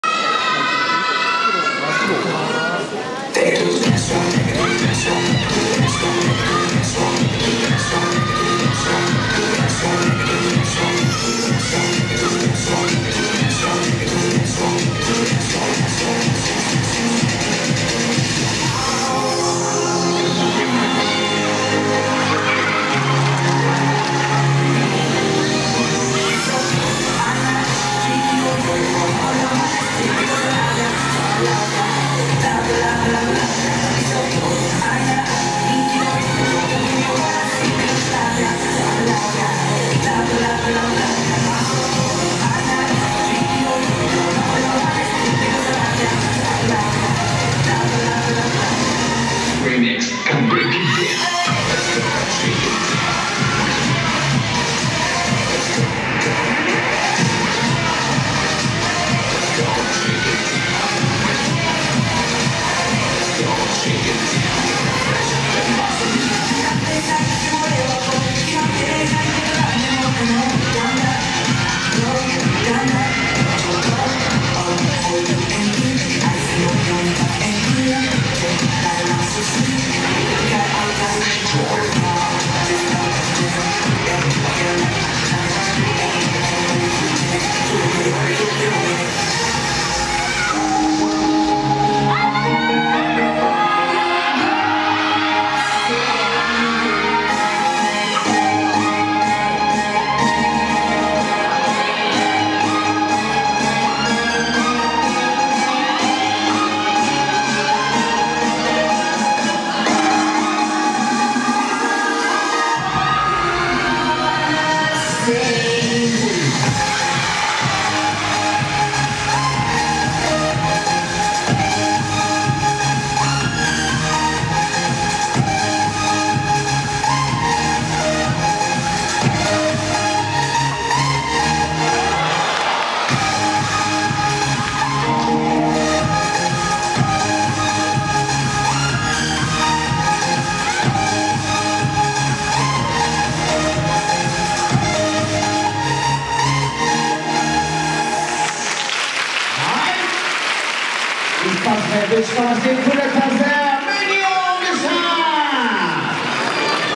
DDCJF2015 minion（照明トラブル）
Double Dutch Contest Japan Final 2015
狛江ダブルダッチクラブ
照明トラブルによりもう一回パフォーマンスをやり直し